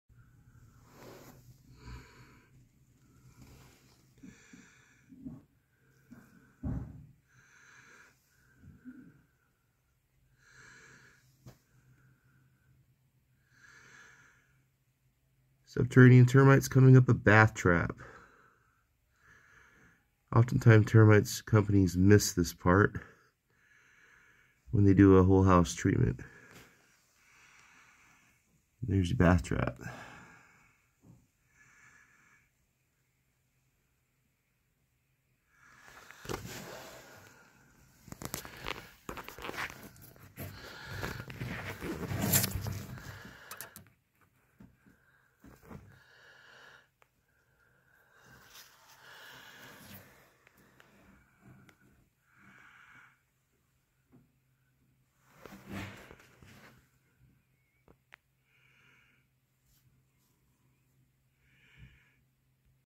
subterranean termites coming up bathtrap under bathtub
often times termite inspectors mis the bathraps when quoting treatments on inspections. here is a large colony of subterranean Termites in a bath trap going up into wall voids. crazy. If you live in Maricopa or Casa Grande AZ contact Specialized Pest Control for your pest needs.